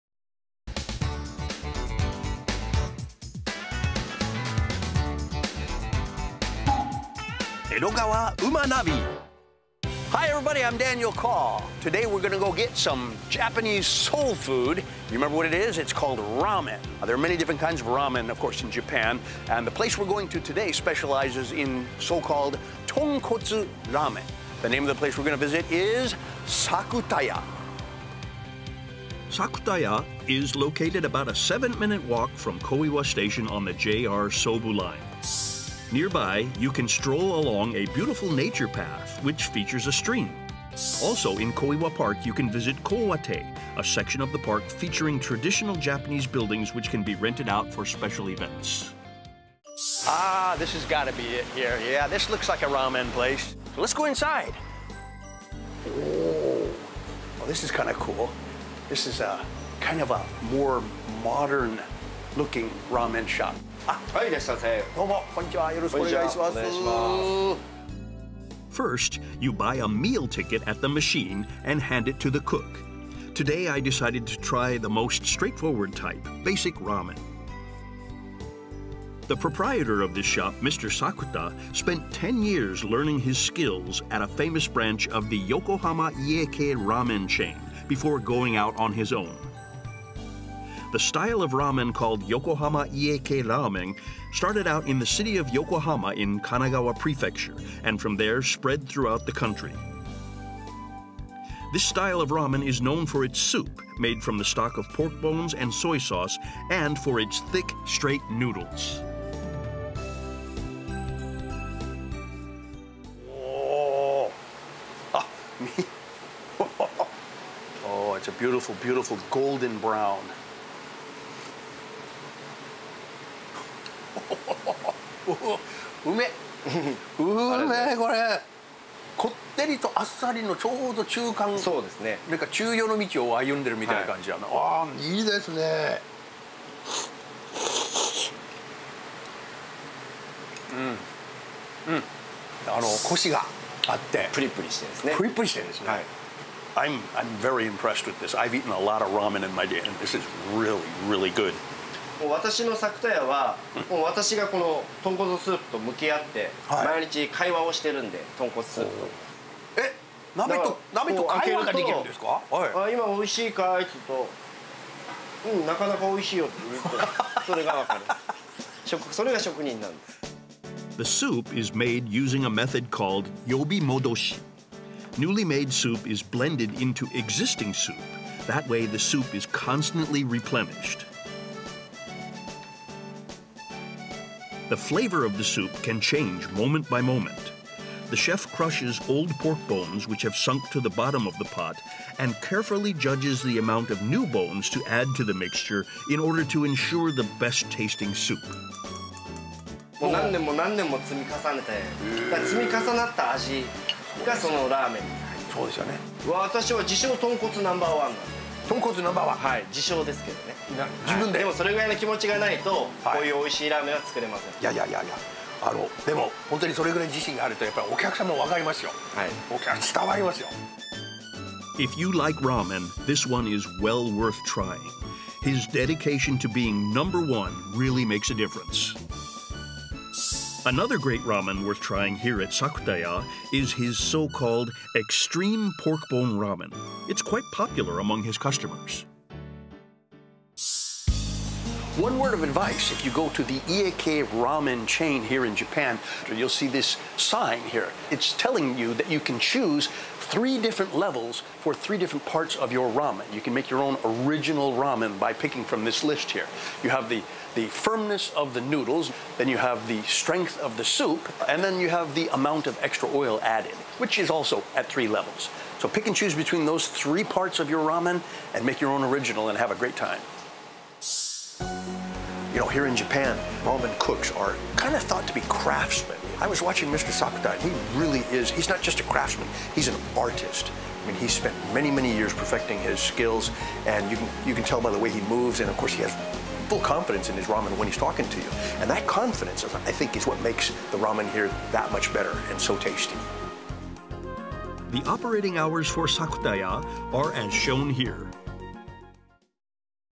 #35 SAKUTAYA【作田家】 EDOGAWA美味NAVI
Nov.2017
Their business hour has been changed as below/
12:00～13:30/18:30～23:30 (Weekday and Saturday)
12:00～14:30/18:30～22:00 (Sunday and Holiday)

2017年11月
営業時間が以下の通り変更になりました。
平日・土曜日　12:00～13:30/18:30～23:30
日曜日・祝日　12:00～14:30/18:30～22:00

~SAKUTAYA~
・SAKUTAYA is a “Ie-kei Ramen” restaurant in Koiwa district of Edogawa city. (Ie-kei Ramen is known for its soup made from the stock of pork bones and soy sauce, and for its thick, straight noodles.)
・Our reporter, Daniel, tries its most straight-forward type, basic Ramen.
・Transportation : About a 7-minute walk from Koiwa Station(JR Sobu Line).
・Address : 1-29-6-1F, Nishi-Koiwa, Edogawa-city, Tokyo

~作田家~
・小岩にある家系ラーメンのお店です。
・お店のスタンダードメニュー「ラーメン」をダニエルさんがリポートします。
・アクセス：JR総武線「小岩」下車、徒歩約７分
・住所：東京都江戸川区西小岩１－２９－６－１Ｆ